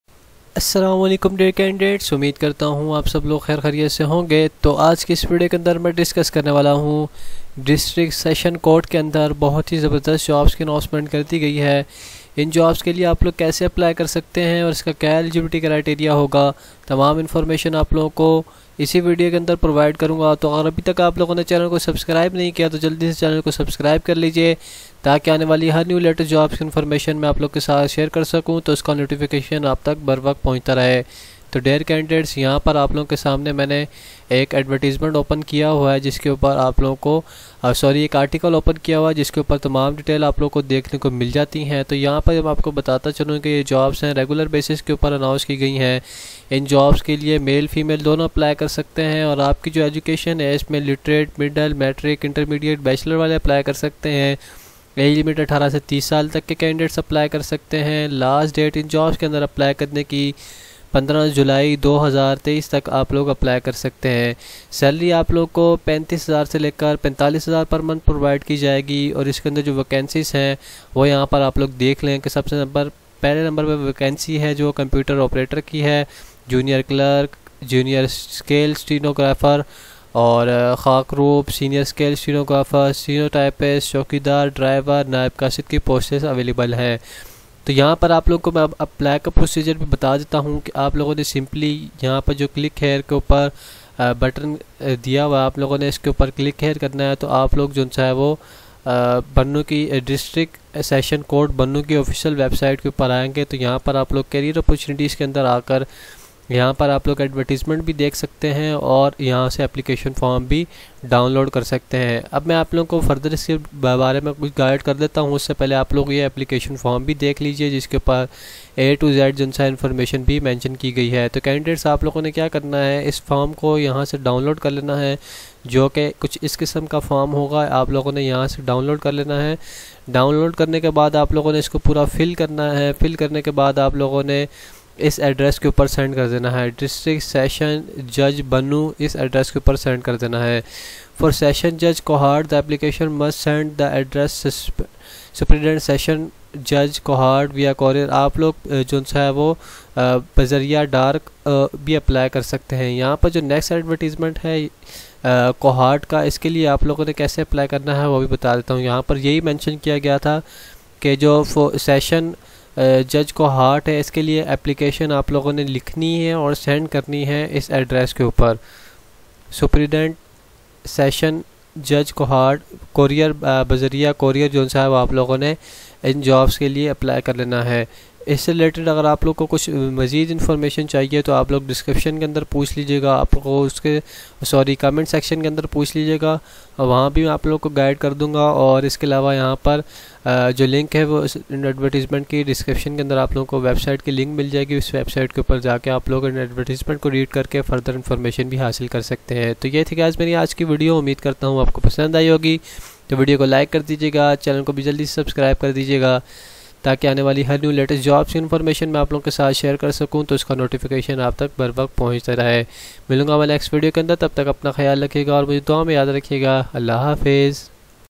distric session court jobs 2023 | session court jobs bannu application form | government jobs 2023
Interested candidates applying for Session court Bannu must download the application form Click Here.
Fill out the application form and send it to address  District & Session, Judge Bannu.
For Session Judge Kohat, the application must send to the address of Superintendent Session Judge Kohat via courier or post office.